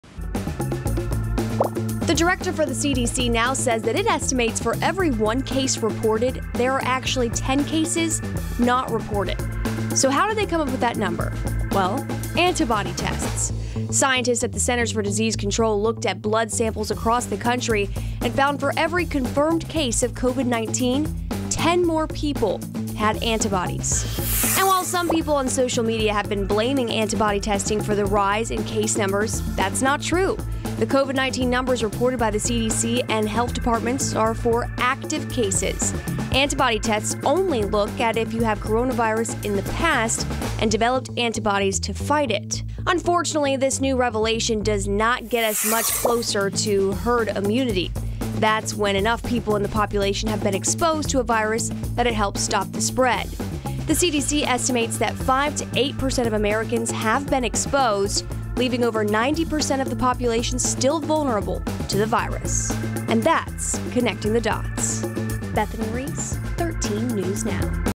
Connect the Dots: CDC says 10 cases present for every 1 reported
The CDC estimated that for every one positive test result, 10 more people have had the virus. They got these numbers from analyzing antibody test data.